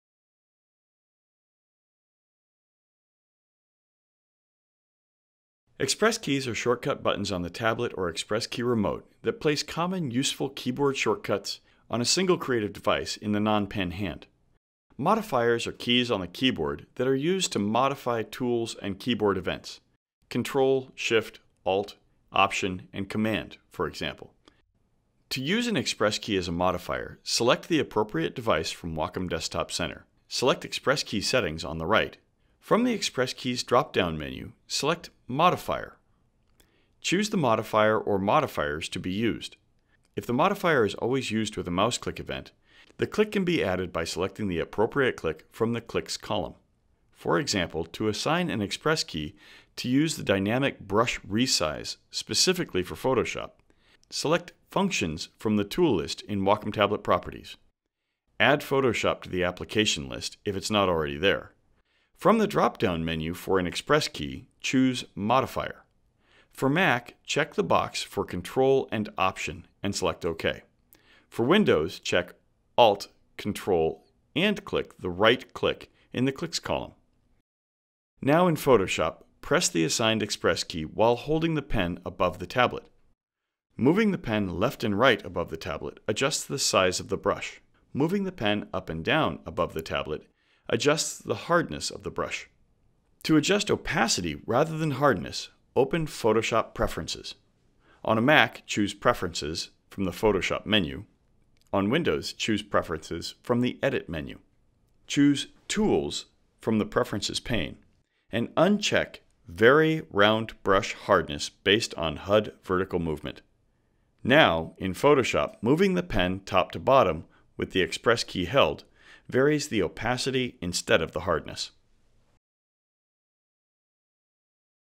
How to Set an ExpressKey to a Modifer
Setting an ExpressKey to a modifier on a Wacom Intuos Pro pen tablet and ExpressKey Remote.